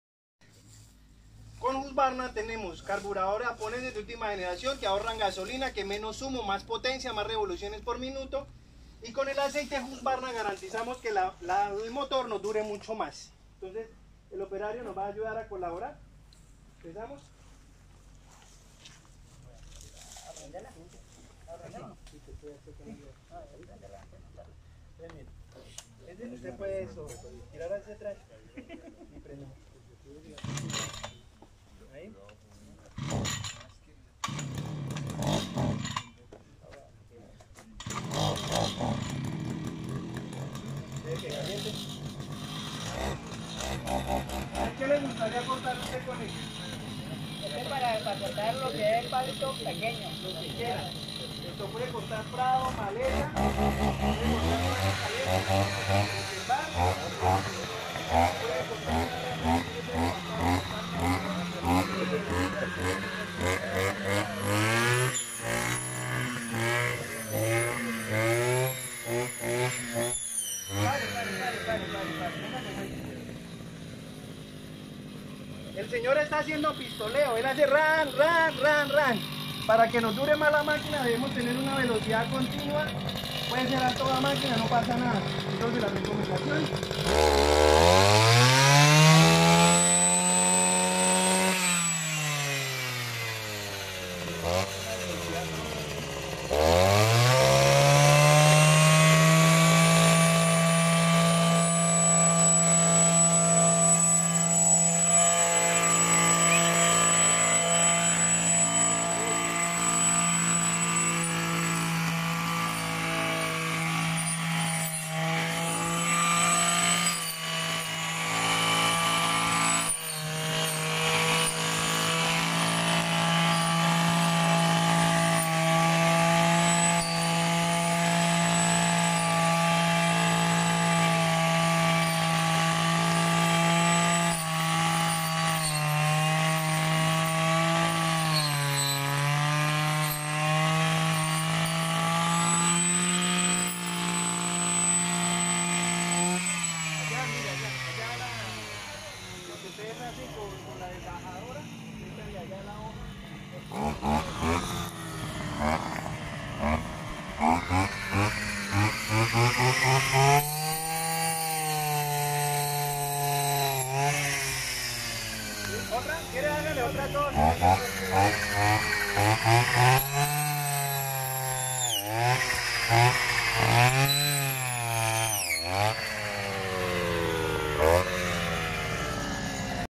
Demostración Guadañadora Husqvarna 541rs con disco multiproposito asociación de Palmeros en campo.
Guadaña 541 rs de Husqvarna con Palma en campo demostración promoción mercadeo y ventas maquinaria agrícola y forestal distribuidores amigos y clientes Santander, Cesar, Boyacá y Norte de Santander.